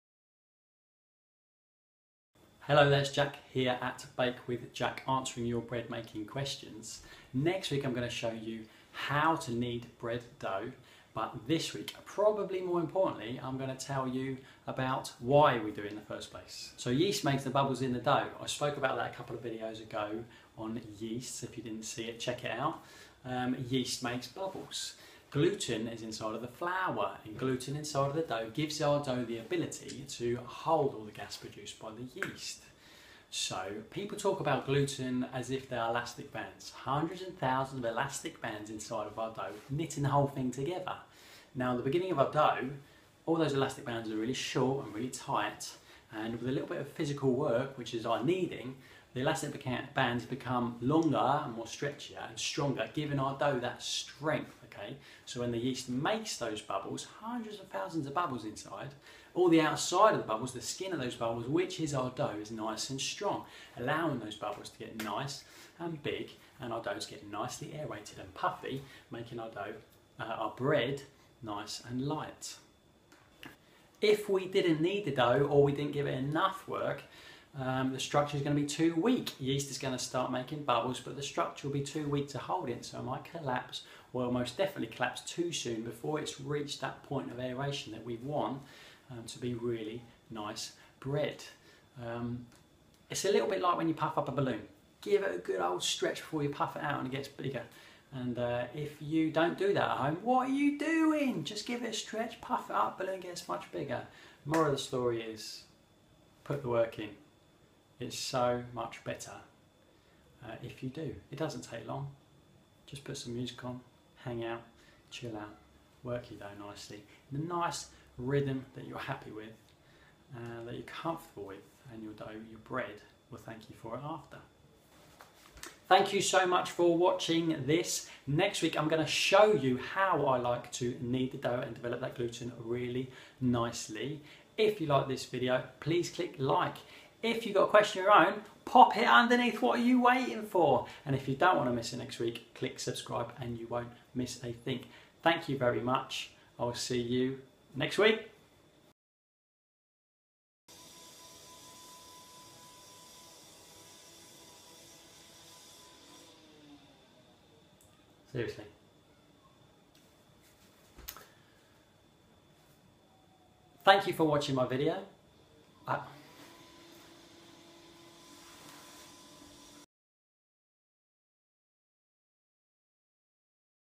5: Why do we knead bread dough? - Bake with Jack
More important than how to knead your dough is why we actually do it in the first place... with a little knowledge you can begin to rule out error when making bread, and make it a roaring success time after time.

Amazon US
Amazon CA